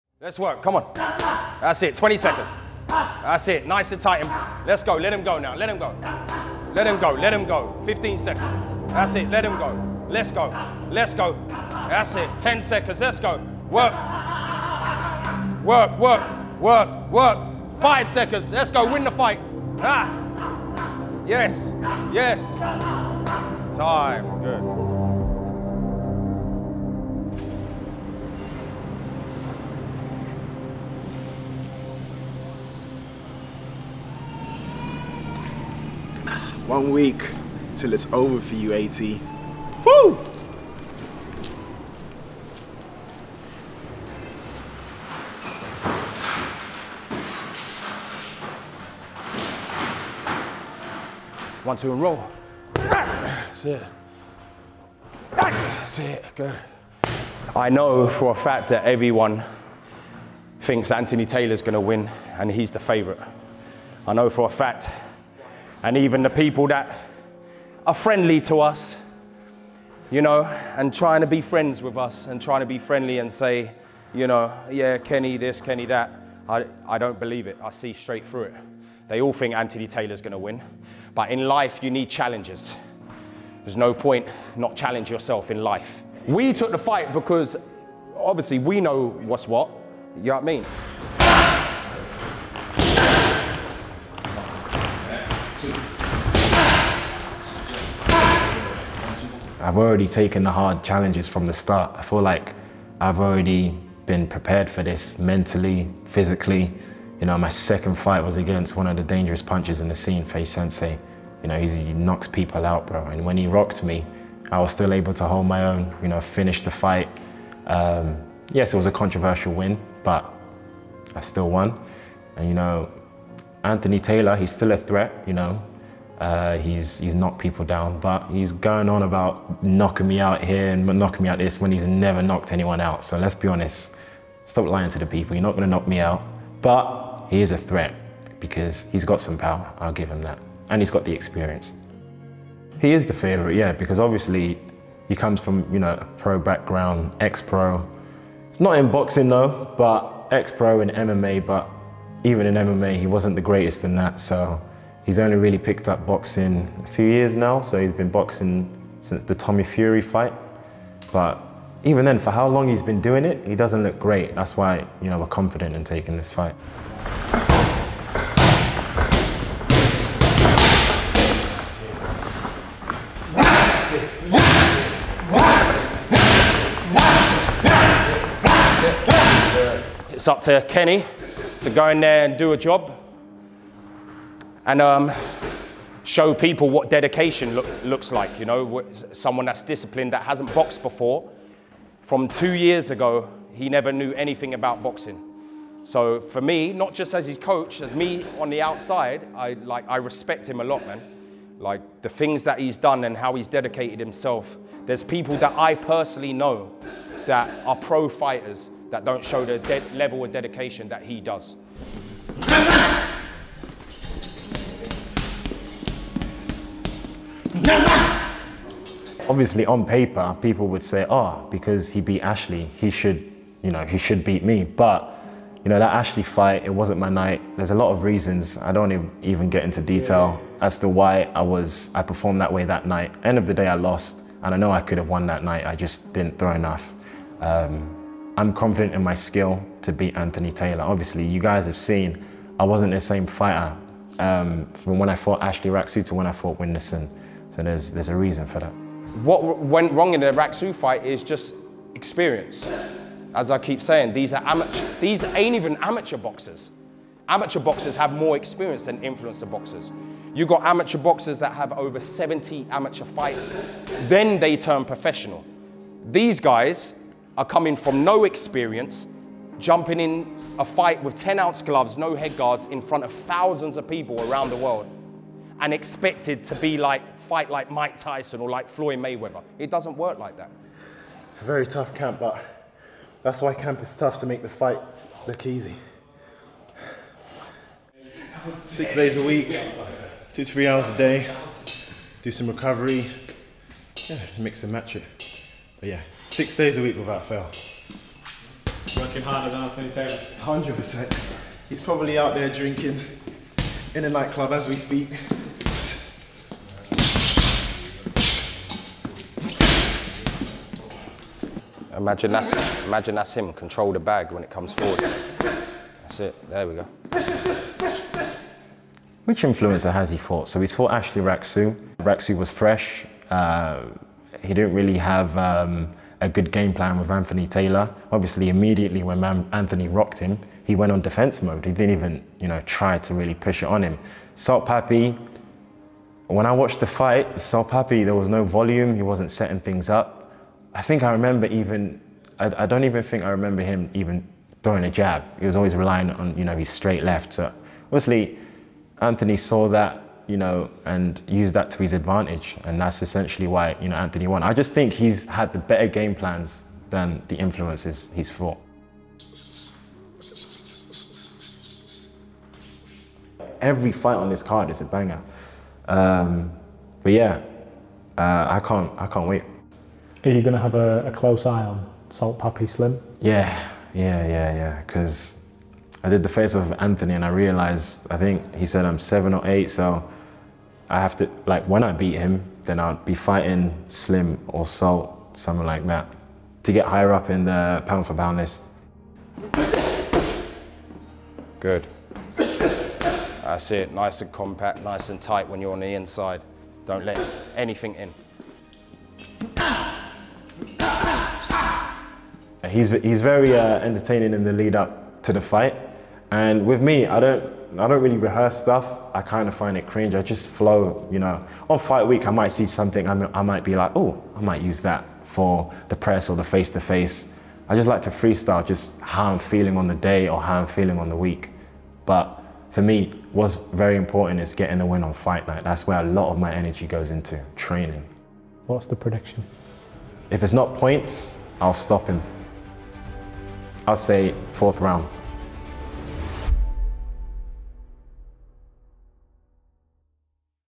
UNINTERRUPTED: KING KENNY - In camp for Anthony Taylor | Misfits & DAZN X Series: The Prime Card
KSIFury is coming at the top of the BIGGEST card this year also featuring Logan Paul v Dillon Danis.